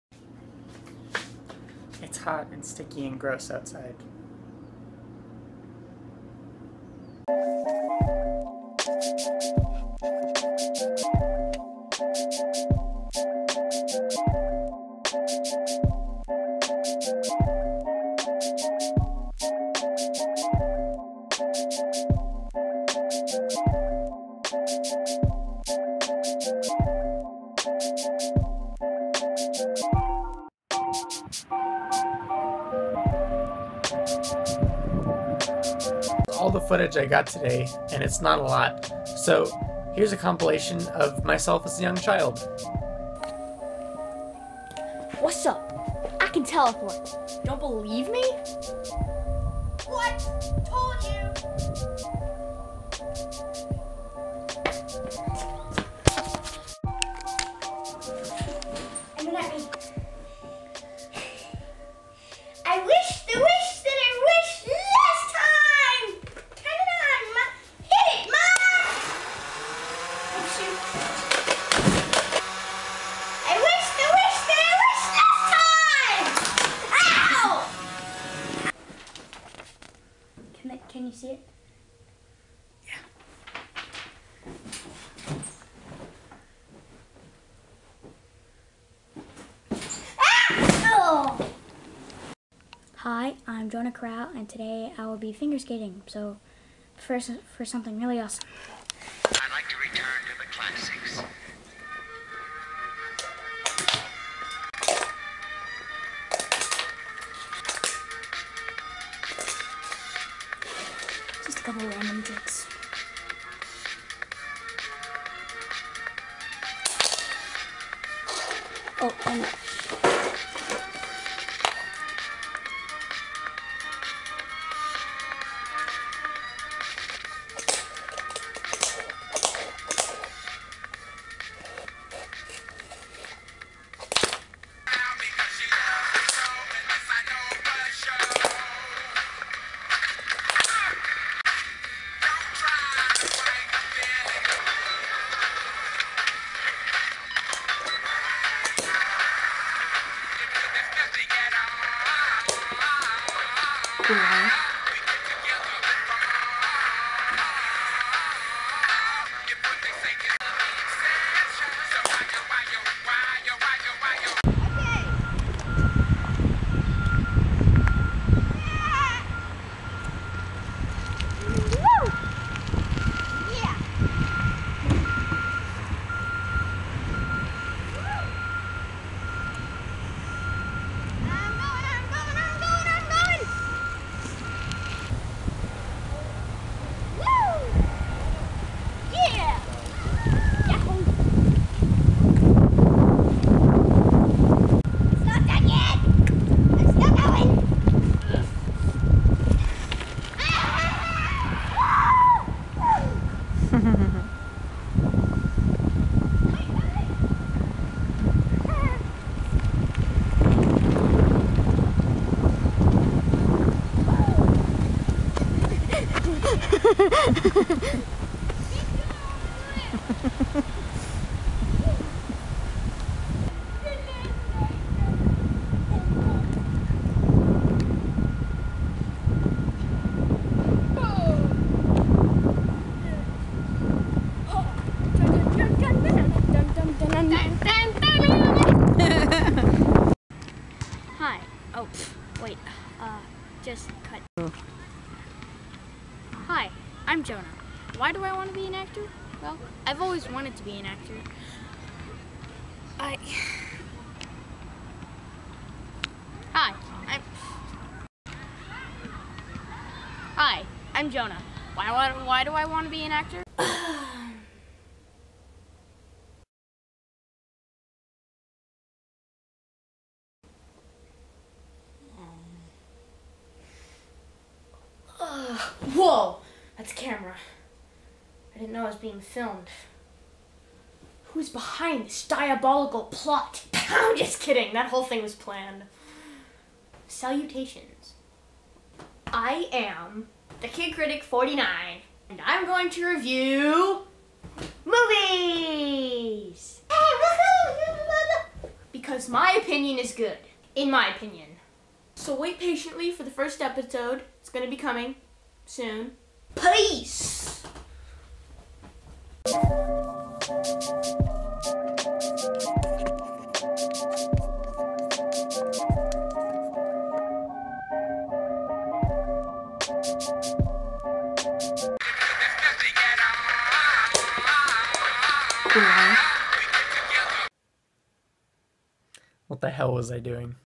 Old footage
I basically didn't film anything today besides two really awesome time lapses, so I pulled out the cringy footage of myself as a lil youngster filmmaker. You're welcome.